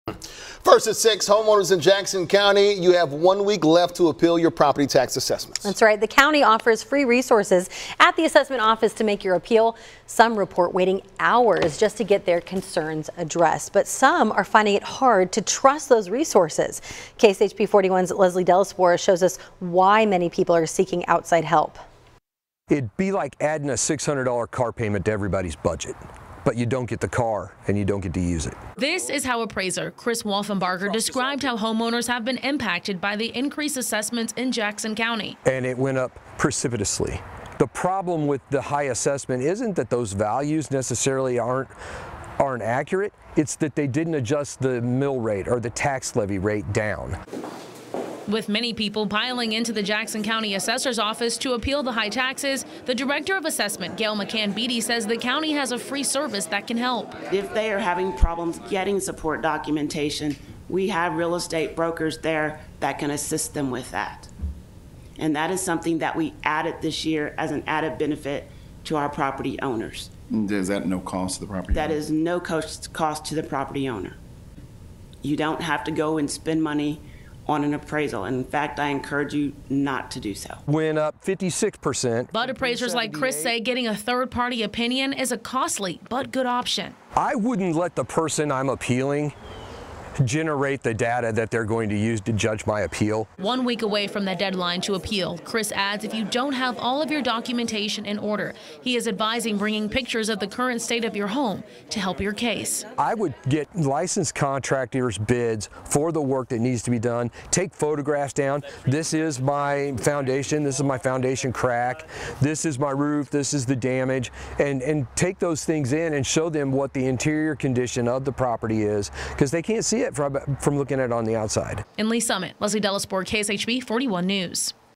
Advice from an appraiser for taxpayers who plan to appeal their property appraisals
Appraiser explains what taxpayers will need to bring with them at their appraisal hearing